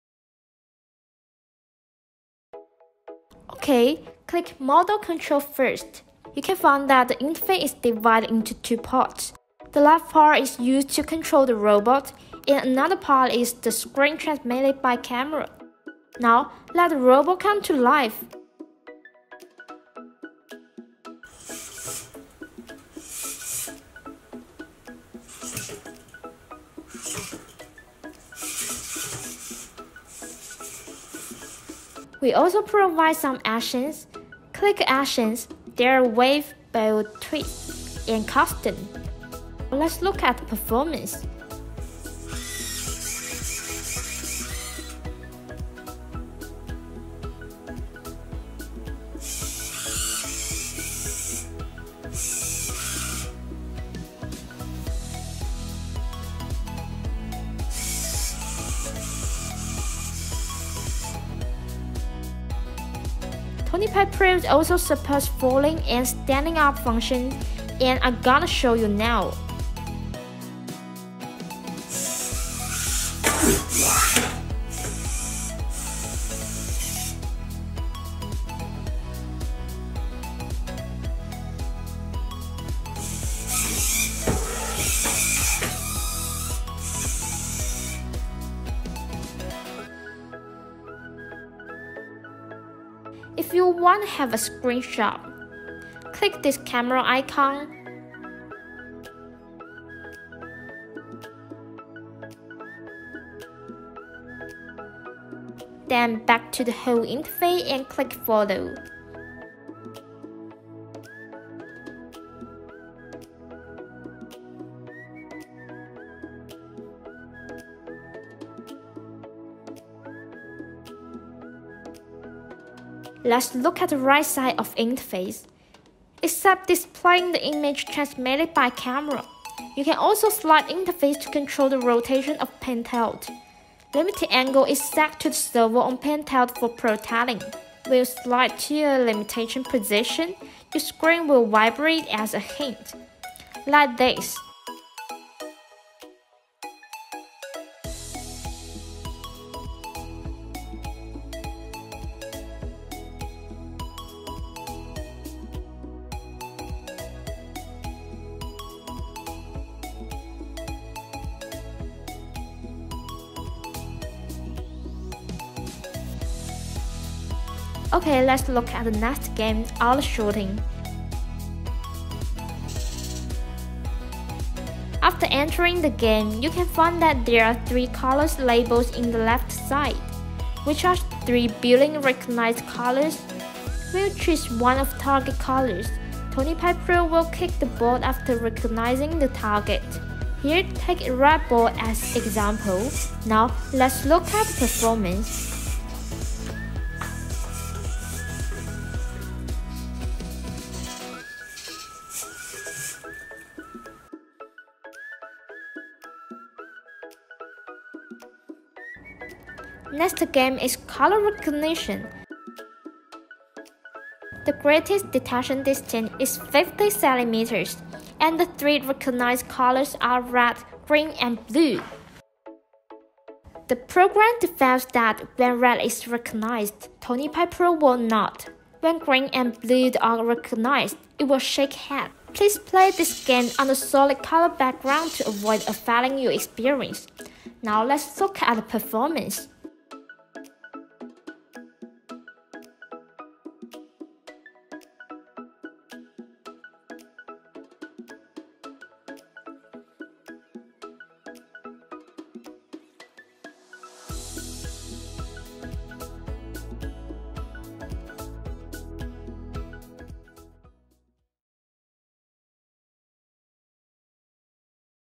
TonyPi Pro Tutorial--Quick User Experience 1
TonyPi Pro: Hiwonder Humanoid Robot Professional Development Kit Powered by Raspberry Pi 4B 4GB
Powered by Raspberry Pi 4B, TonyPi Pro is an AI vision humanoid robot development kit with many skills. Based on Hiwonder Tony Pi kit, TonyPi Pro is upgraded to a higher level with more AI games, including clearing hurdles, going up and down stair, intelligent picking, mask recognition, group controlling, etc.